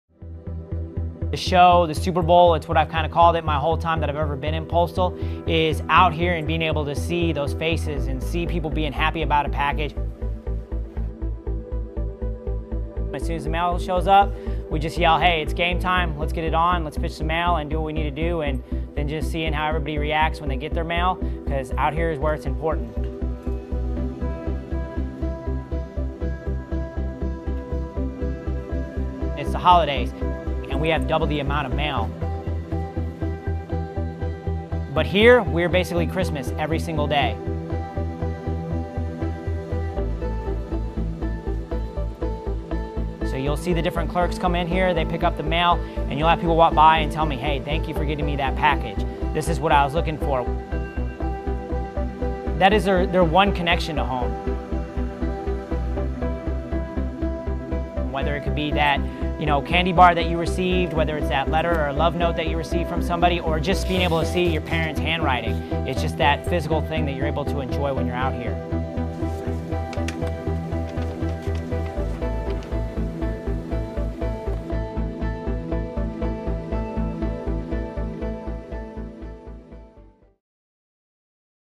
Mail Matters
It's not easy being away from home.

That's why the Marines with Combat Logistics Detachment 5, Special Purpose Marine Air-Ground Task Force - Crisis Response - Central Command, ensure Marines and Sailors forward deployed in the U.S. Central Command area of operations receive mail service in every clime and place.

(U.S. Marine Corps video by Cpl. Jocelyn Ontiveros)